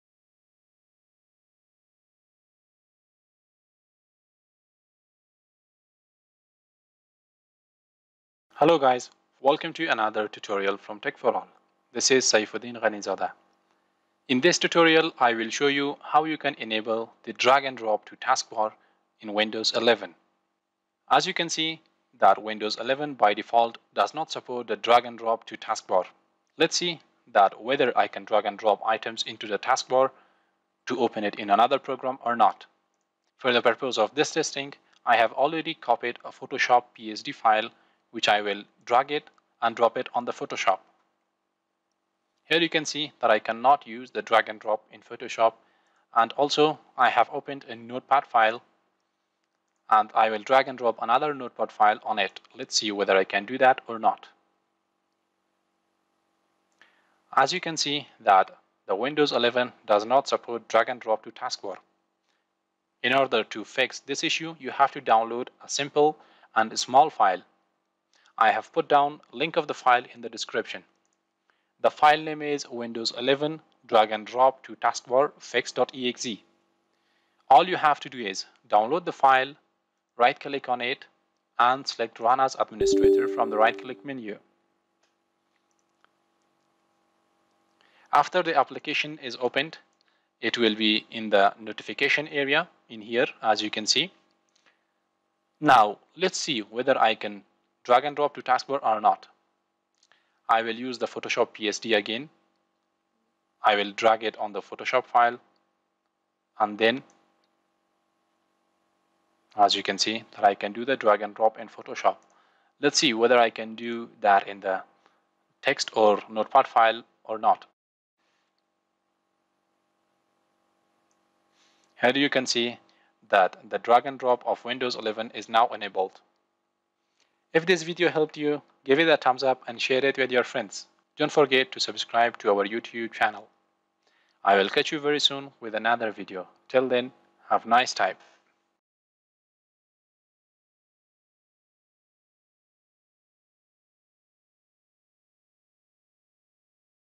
How to Enable Taskbar Drag and Drop in Windows 11?
Hey Guys,
As you guys know that while Windows 11 was released first it had some issues and Microsoft fixed some of those issues with Windows update but one of those problems which is the Drag & Drop to Taskbar is still not fixed. In this video, I will show you guys how you can fix this issue.

𝔽𝕚𝕟𝕕 𝕄𝕠𝕣𝕖 𝕍𝕚𝕕𝕖𝕠𝕤 𝕠𝕟 𝕎𝕚𝕟𝕕𝕠𝕨𝕤 𝟙𝟙

𝔽𝕚𝕟𝕕 𝕄𝕠𝕣𝕖 𝕍𝕚𝕕𝕖𝕠𝕤 𝕠𝕟 𝕎𝕚𝕟𝕕𝕠𝕨𝕤 𝕊𝕖𝕣𝕧𝕖𝕣 𝟚𝟘𝟚𝟚